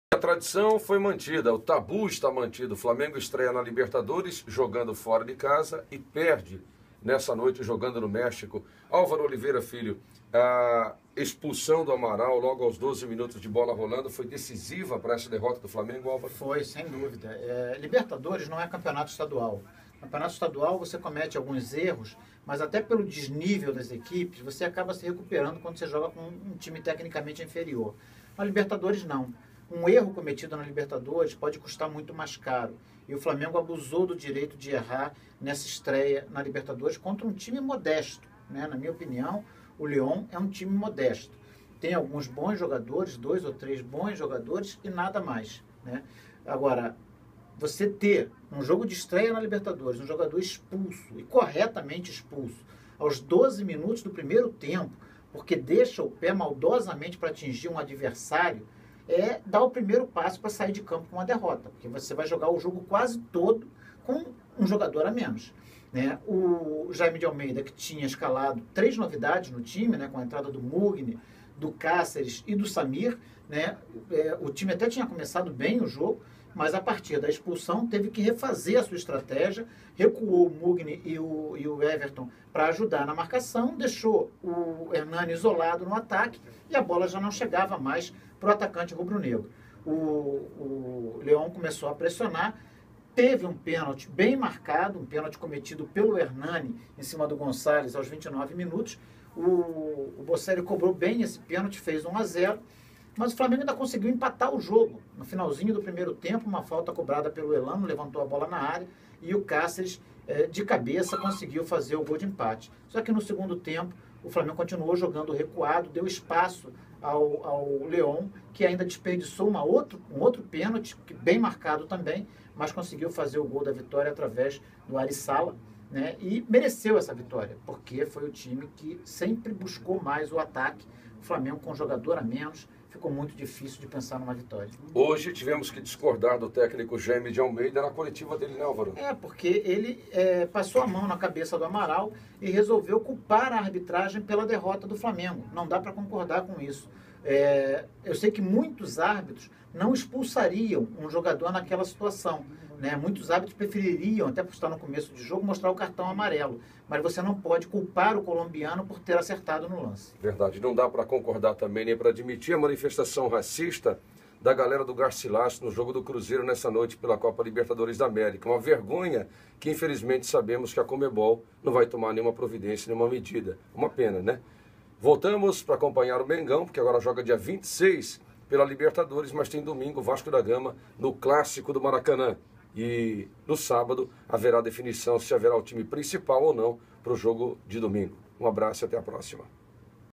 Fla estreia com derrota no México
Evaldo José e Alvaro Oliveira Filho analisam a partida em que o time rubro-negro perdeu para o Leon por 2 a 1. Comentarista CBN diz que expulsão de Amaral logo no início foi determinante. Você concorda?